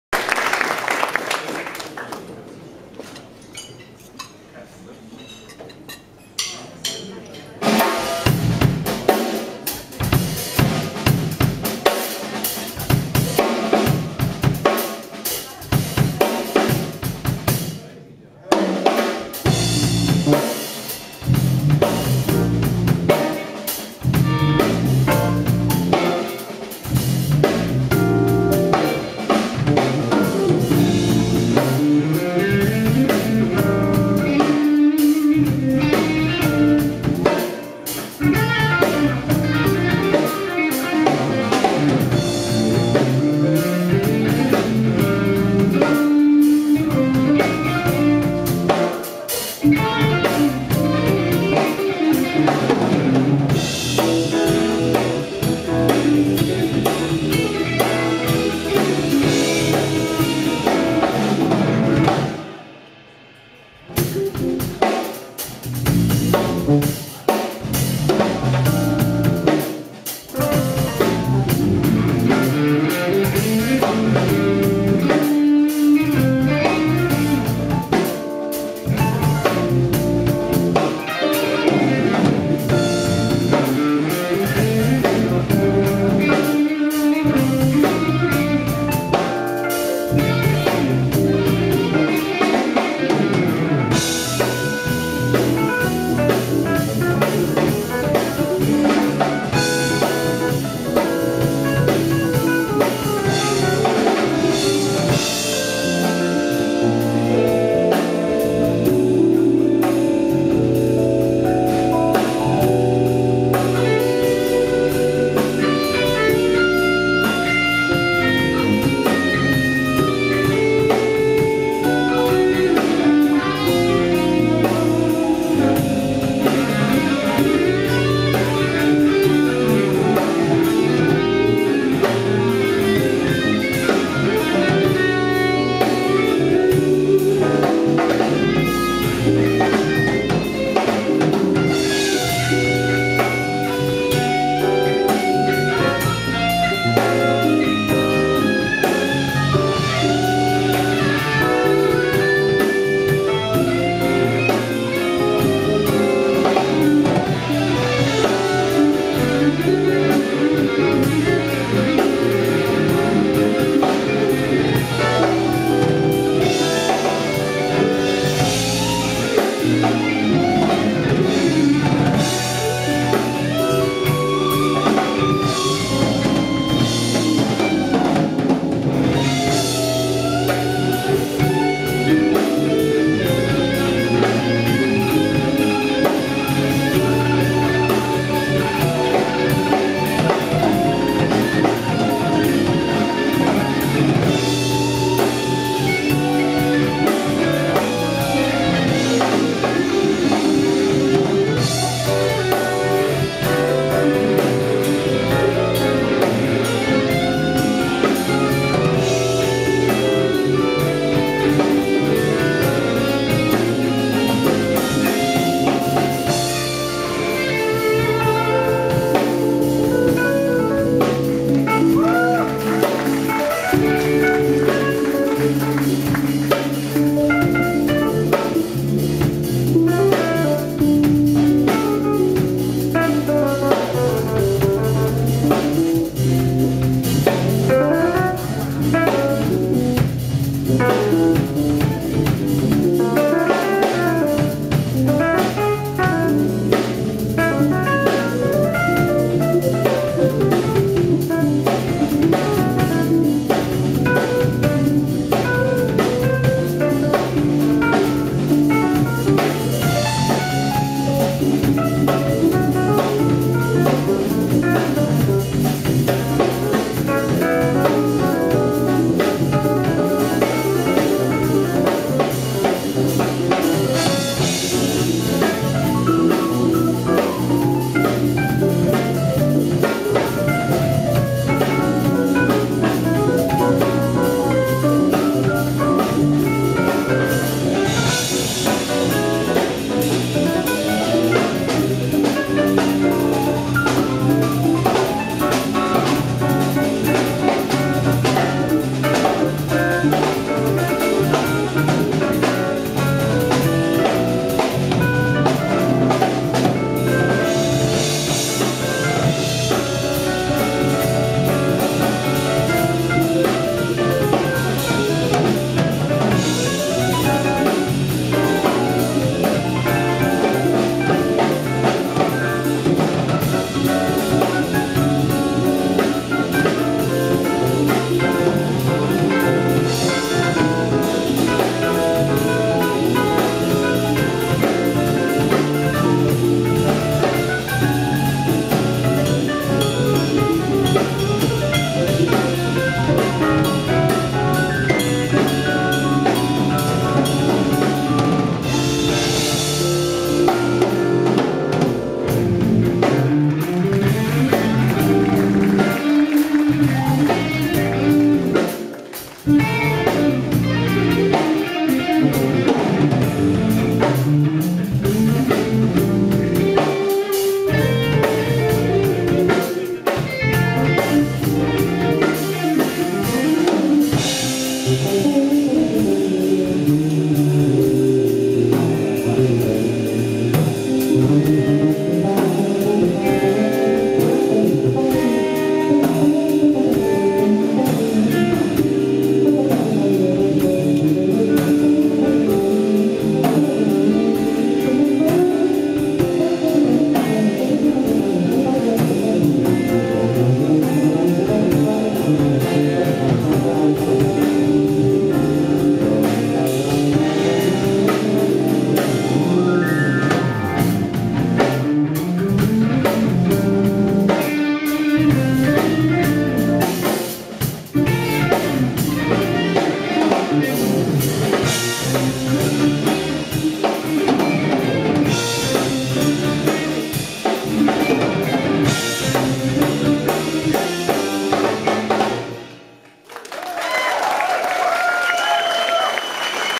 [HD] CAB - Tony MacAlpine, Virgil Donati @ Live in Moscow 2010. part 2
CAB - Tony MacAlpine(guitar), Virgil Donati (drums), Bunny Brunel (bass), Dennis Hamm (keyboards) @ Live in Moscow. part 2. Igor Butman's Club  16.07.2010